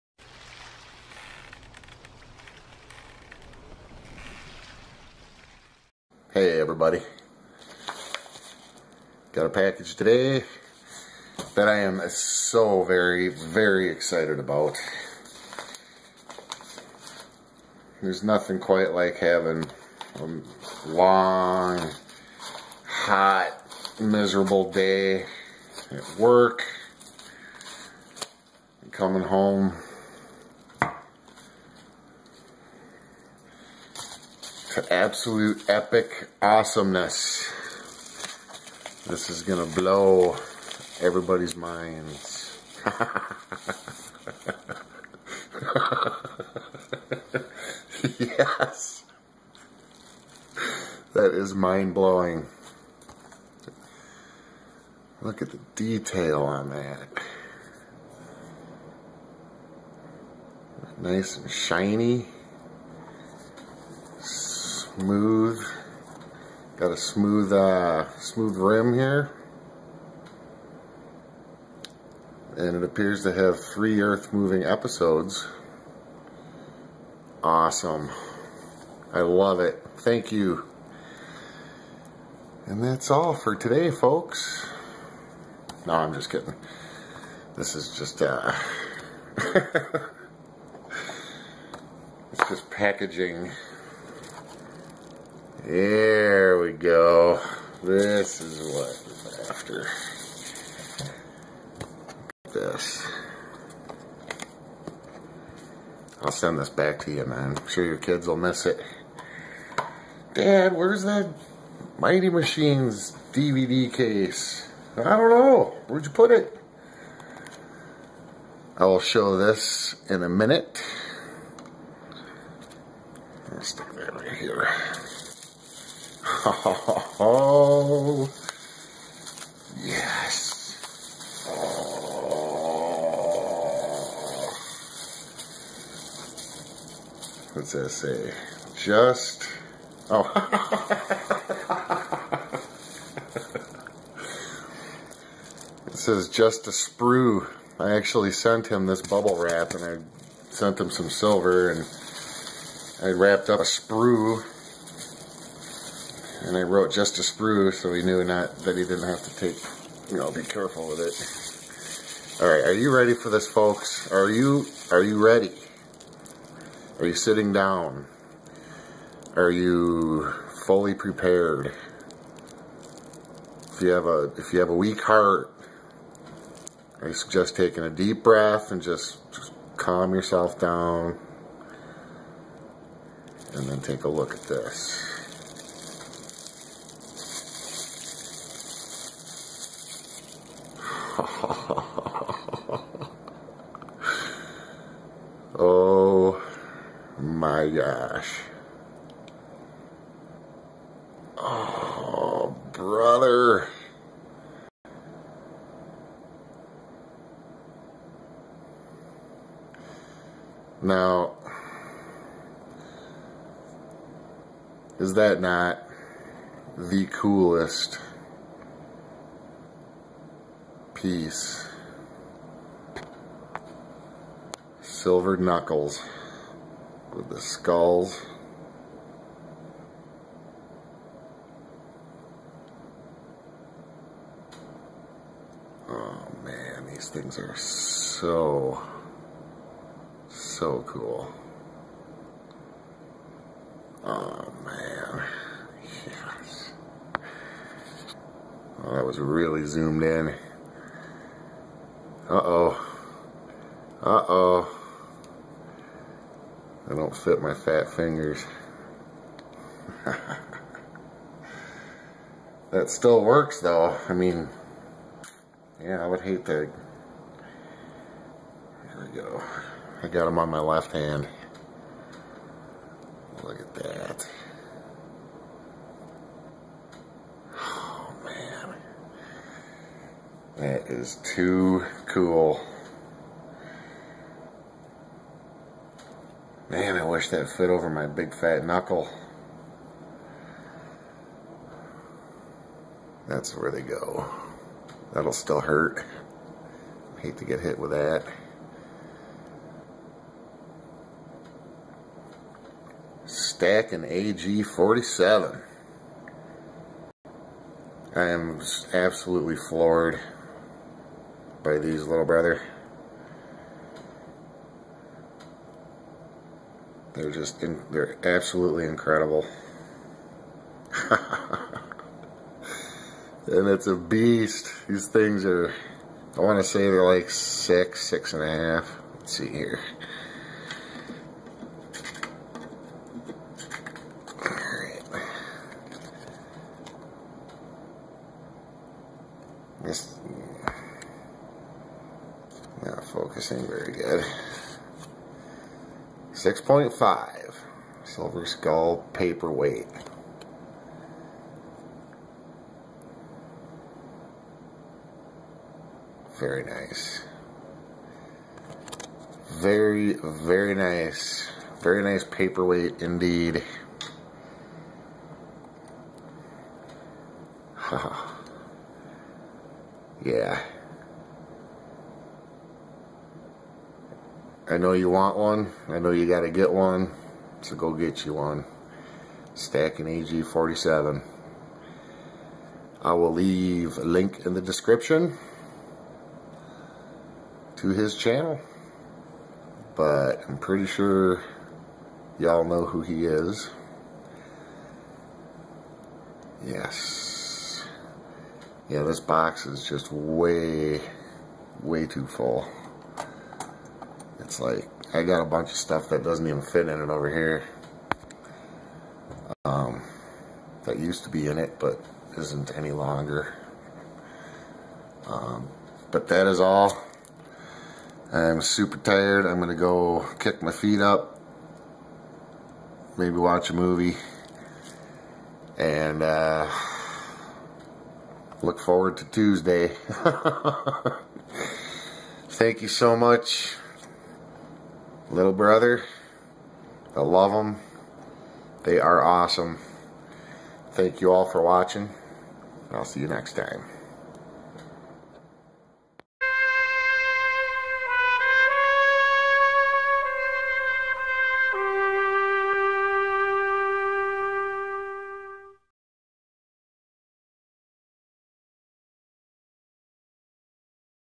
Silver Unboxing - Epic Stackin Ag47 Knuckles
Thank you Clinty 💗 They are so bad ass!! Love ya little brother!

Here is a link to the making of these bad boys! Check-in out!